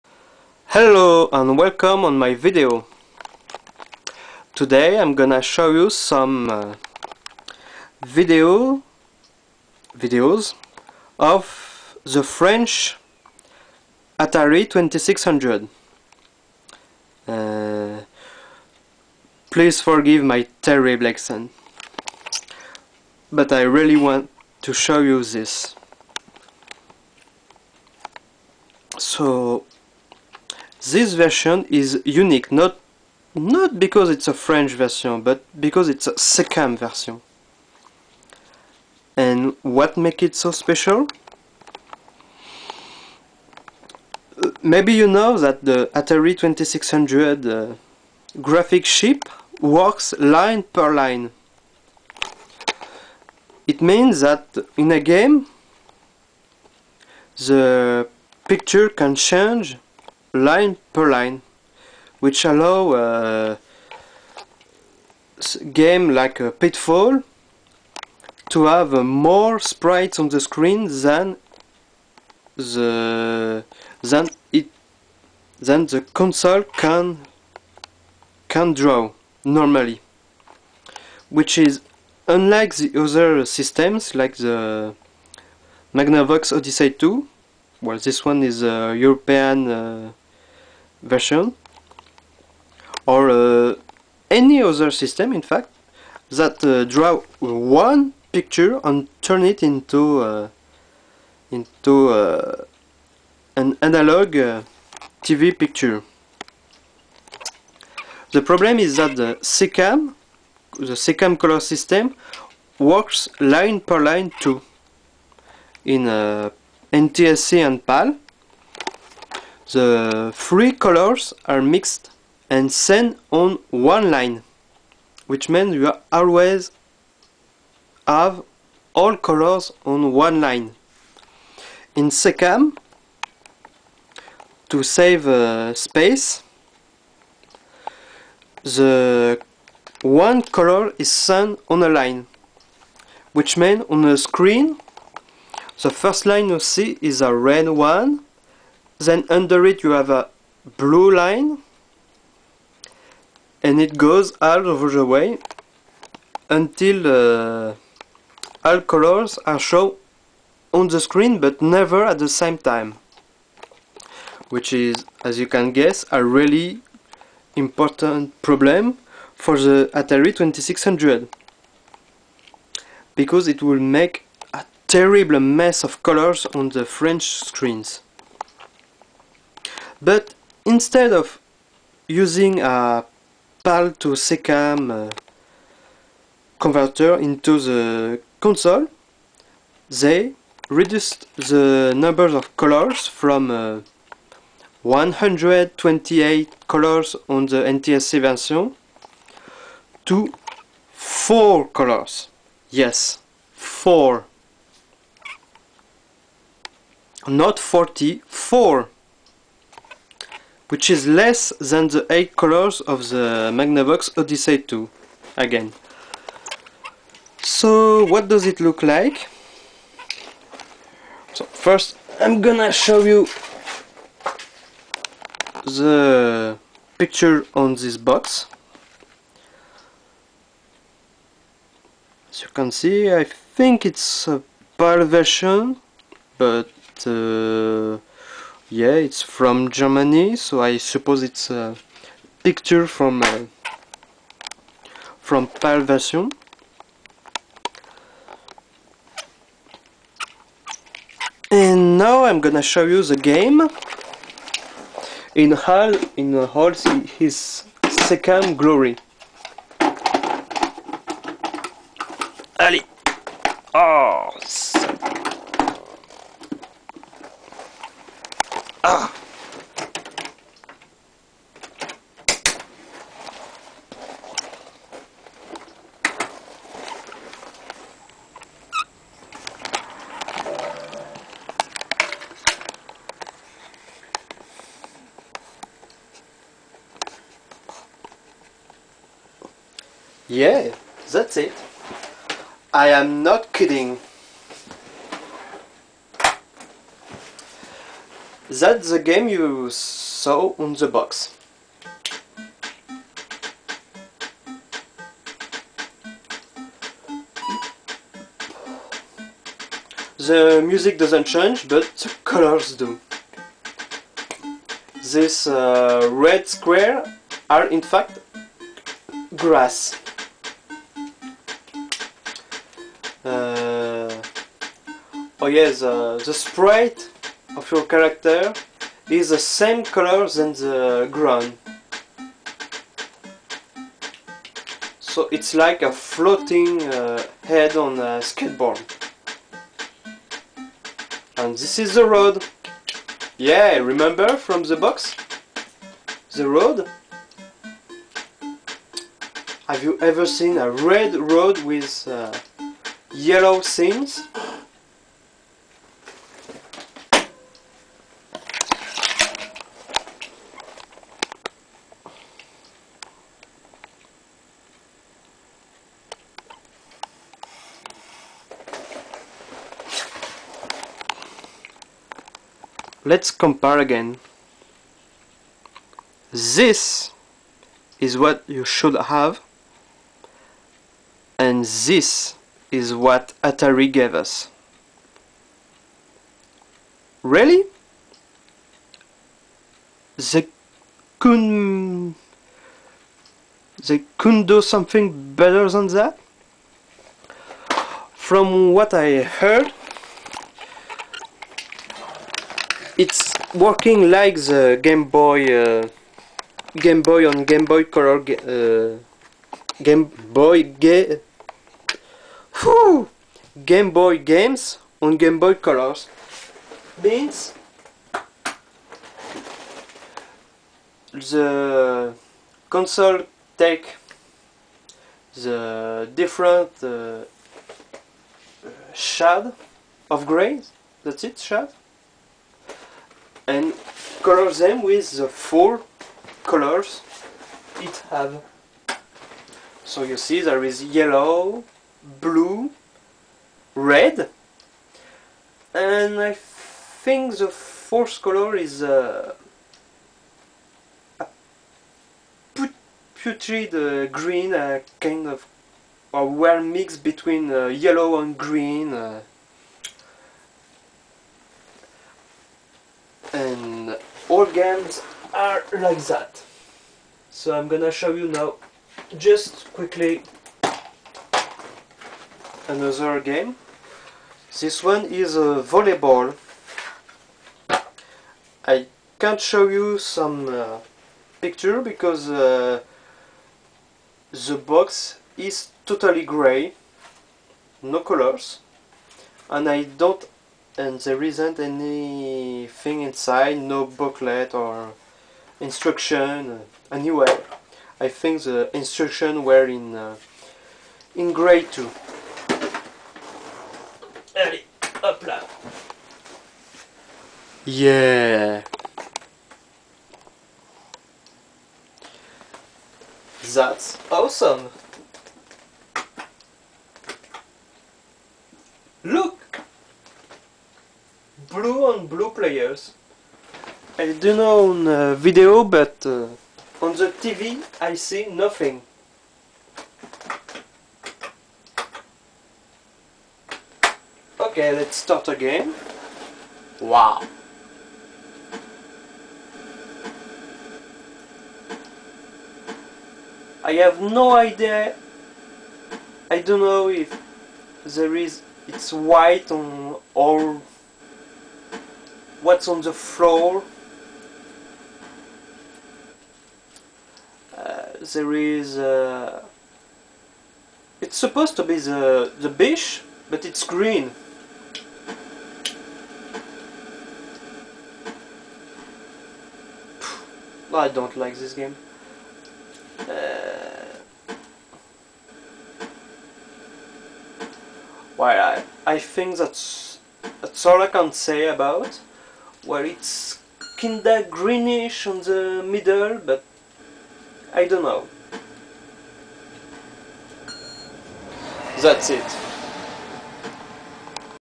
RetroTest - Atari 2600 SECAM french model
I'm French, so forgive my terrible accent. ^^'
A little test of the Atari 2600, french SECAM version.
Released only in 1981, the 2600 never really became a "cult classic" video game system for French gamers.
For many reasons; the first being the very late release on the market, while the Odyssey² was on the market since 1978, and first cheap computers like the Oric Atmos already coming.
Second reason, the price. The 2600 was almost twice the price of an Odyssey². And the same price than a cheap computer.
Third, the graphics. As I explain on this video...